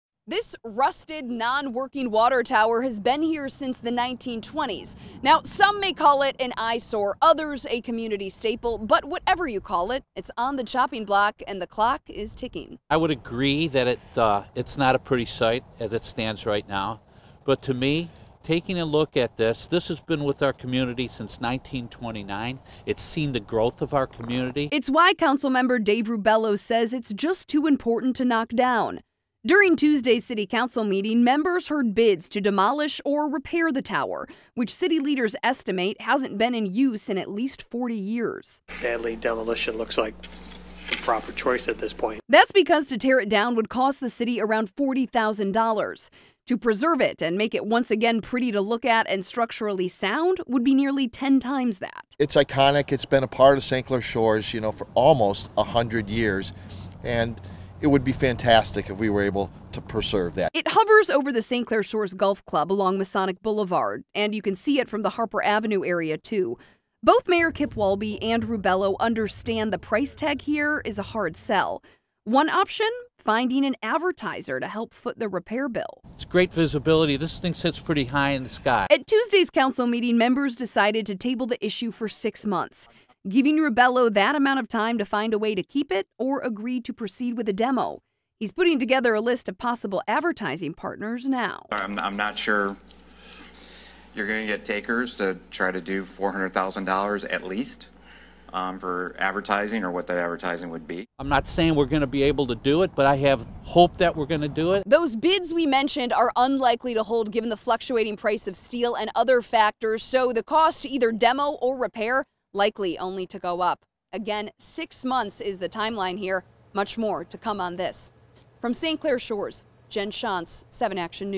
Push to save historic water tower in St. Clair Shores
Push to save historic water tower in St. Clair Shores; councilman fighting to keep it.